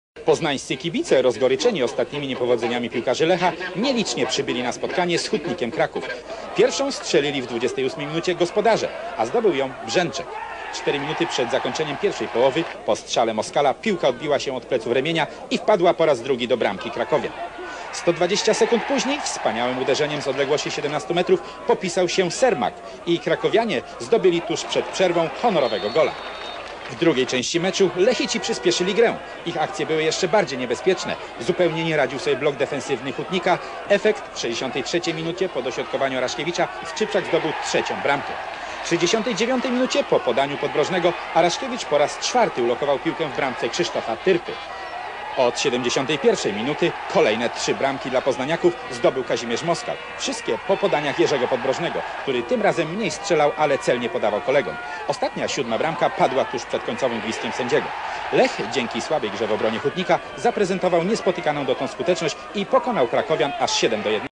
14-15 listopada 1992, Lech Poznań 7-1 Hutnik Kraków
16 kolejka polskiej I ligi w piłce nożnej - 14-15 listopada 1992

LECH POZNAŃ 7-1 HUTNIK KRAKÓW

gole:

1-0 Jerzy Brzęczek 28'
2-0 Ryszard Remień 41'
2-1 Andrzej Sermak 43'
3-1 Dariusz Skrzypczak 63'
4-1 Jarosław Araszkiewicz 69'
5-1 Kazimierz Moskal 71'
6-1 Kazimierz Moskal ?
7-1 Kazimierz Moskal 92'